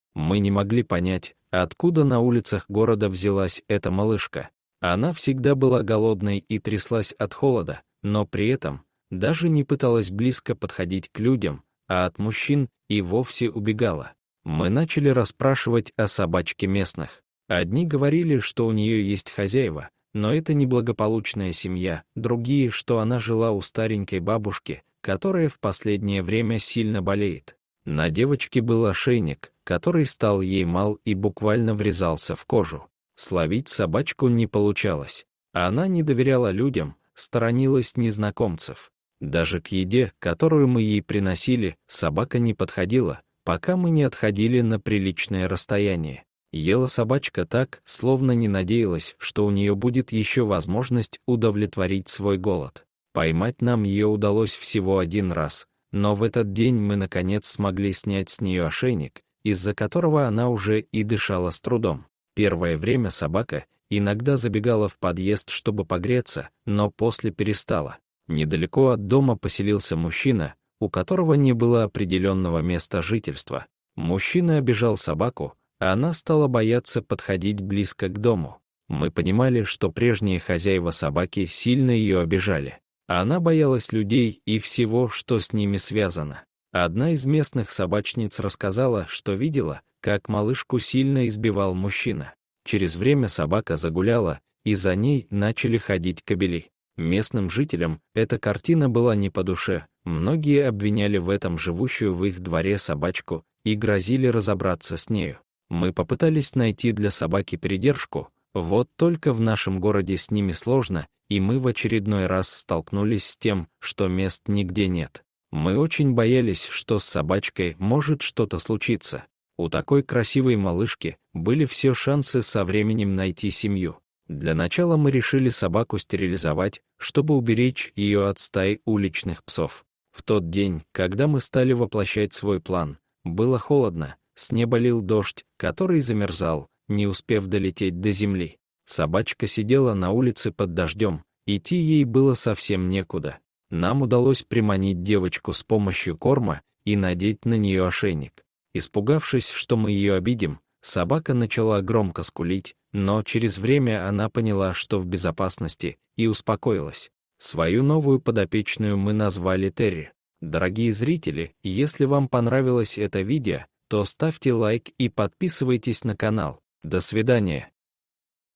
Песик сидел под дождем и плакал, ему некуда было идти. Но случилось невероятное!
Мы не могли понять, откуда на улицах города взялась эта малышка. Она всегда была голодной и тряслась от холода, но при этом, даже не пыталась близко подходить к людям, а от мужчин и вовсе убегала.